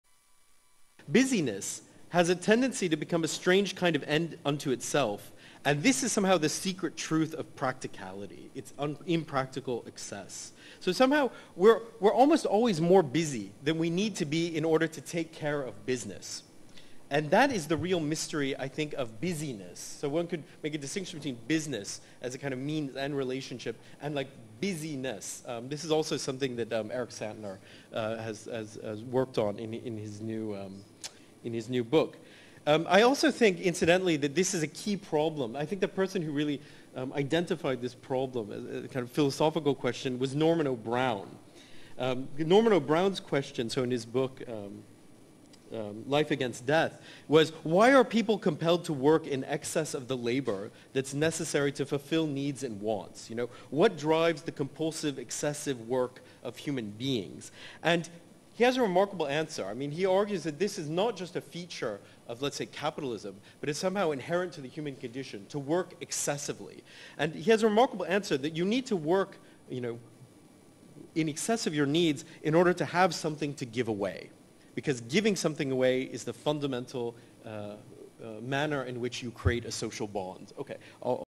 Aaron Schuster – Why we need to work in excess of our needs
"Busyness has a tendency to become a strange kind of end in itself. And this is somehow the secret truth of practicality. Its impractical excess. So somehow we're almost always more busy than we need to be in order to take care of business."